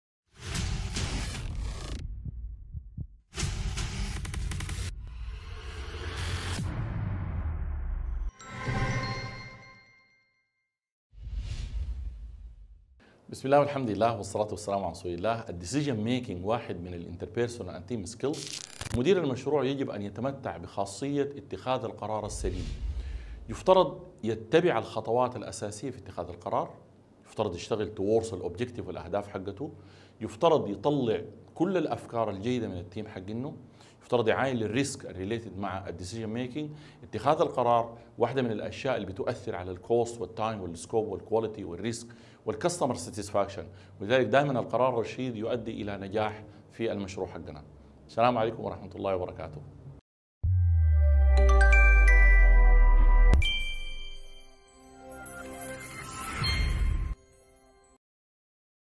Decision Making | 1Min PMP
صنع القرار Decision making  الأساليب المستخدمة لاختيار مسار عمل من عدة بدائل مختلفة ومن أساليبه الإجماع أو الأغلبية أو التعدد.

Decision-making, in this context, involves the ability to negotiate and influence the organization and the project management team rather than the set of tools described in the decision-making toolset. Some guidelines for decision-making include: Focusing on goals to be served, Following a decision‐making process, Studying the environmental factors, Analyzing available information, Stimulating team creativity, and Accounting for risk.